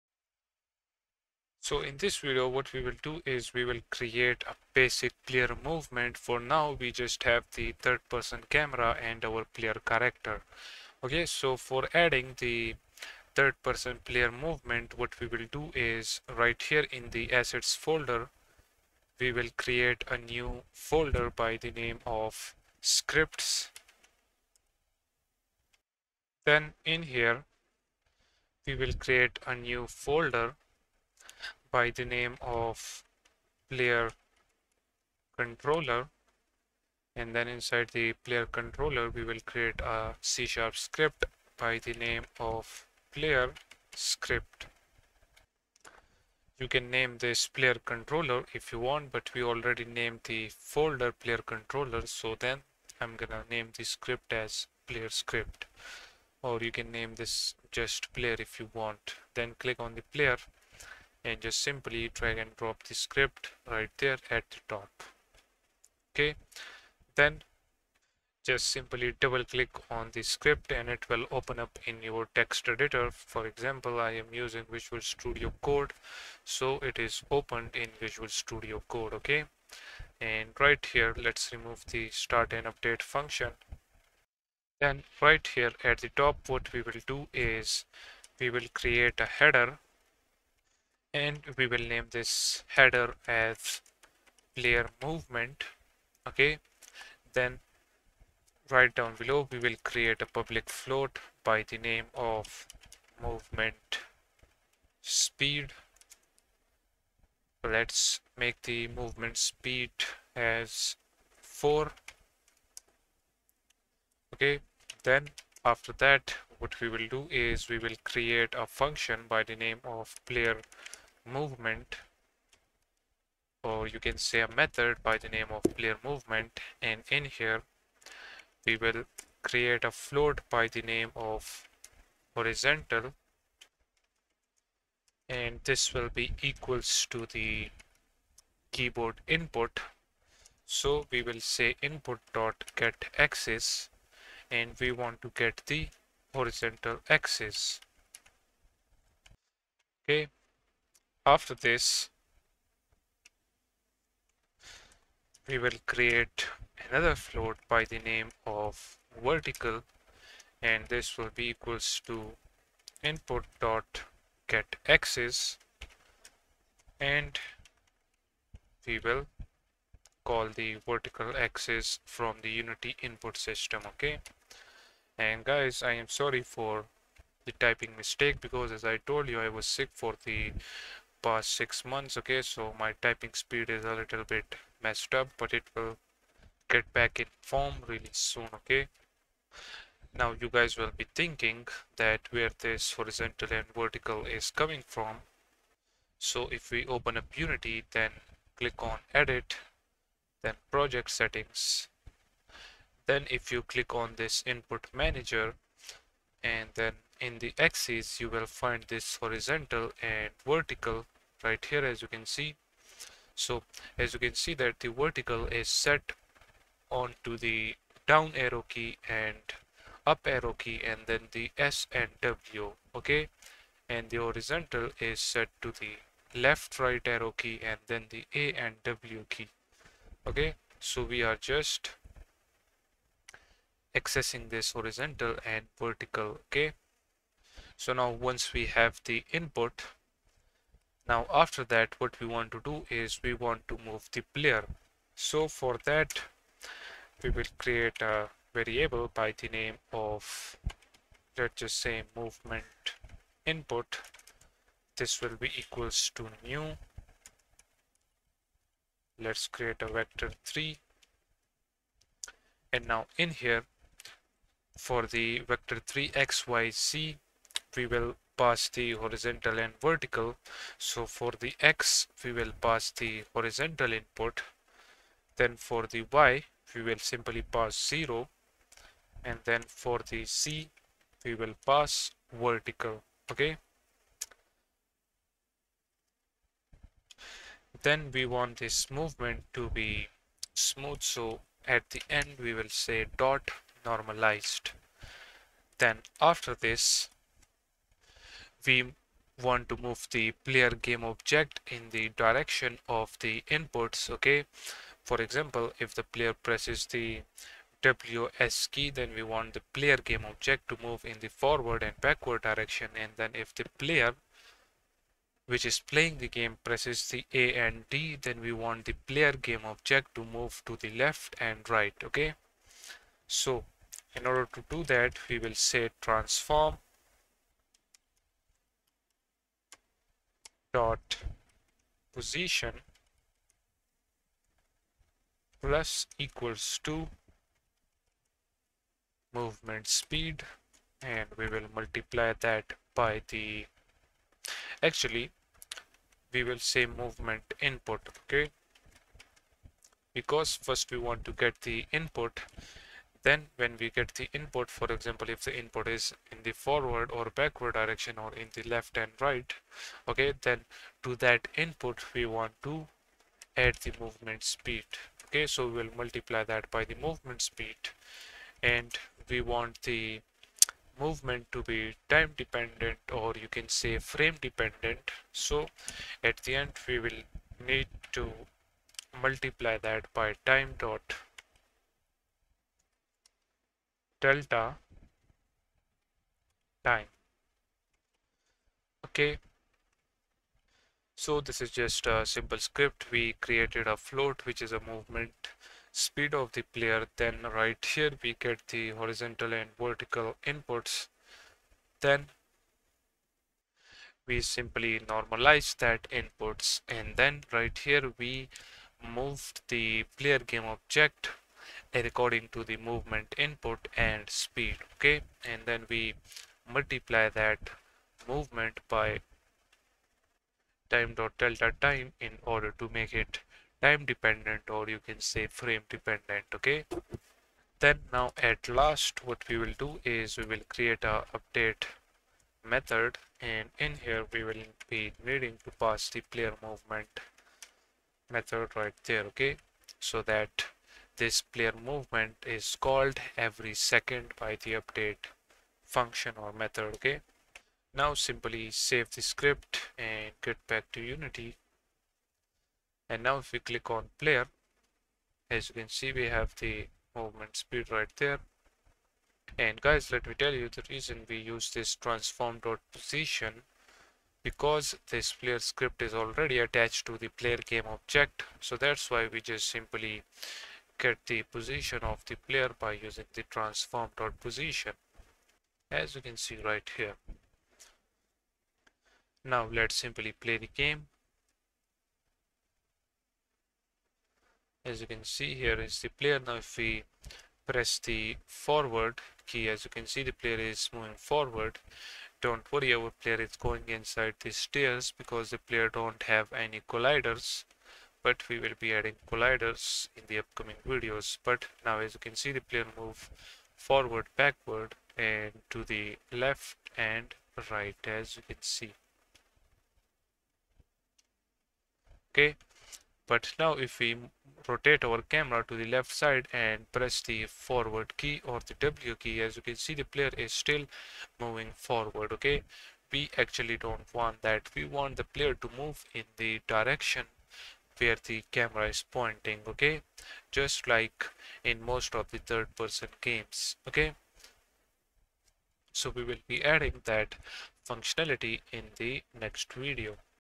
Unity Player Movement | Third Person Controller | Parkour System 3d Game Development Tutorial 07
This is tutorial# 07. In this course your will learn dynamic parkour system using unity game engine. You will learn about detecting obstacles and according detected obstacles height perform different parkour action and parkour jumping that automatically perform parkour action based on obstacle height. You will learn about rotating towards the obstacles, vault action, climb up action and many other custom actions. So you will also learn how to detect ledges, jumping from ledges, ledge movement and much more. You will learn how to climb tall building in unity that is climb ledge detection, then jump to ledge, jump between ledges, how to perform shimmy actions, jump back and climb up from hanging state and how to drop to hanging state. In this complete series you will learn all these things step by step. So please watch each video tutorial step by step.